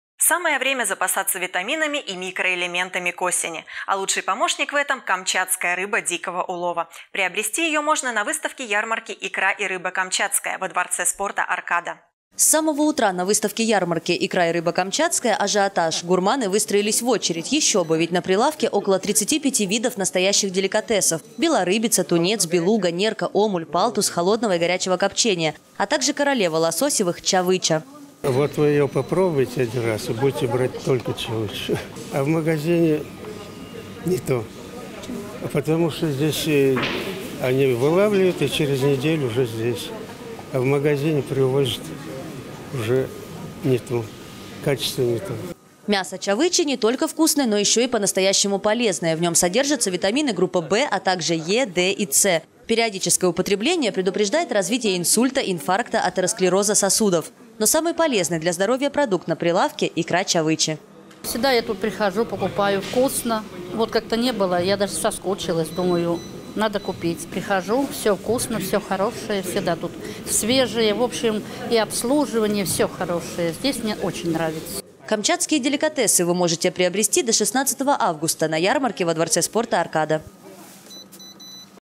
Камчатская рыба дикого улова - снова на ярмарке во дворце спорта "Аркада"!
С самого утра на выставке-ярмарке «Икра и рыба камчатская» ажиотаж. Гурманы выстроились в очередь, еще бы, ведь на прилавке около 35 видов настоящих деликатесов: белорыбица, тунец, белуга, нерка, омуль, палтус холодного и горячего копчения, а так же королева лососевых - чавыча.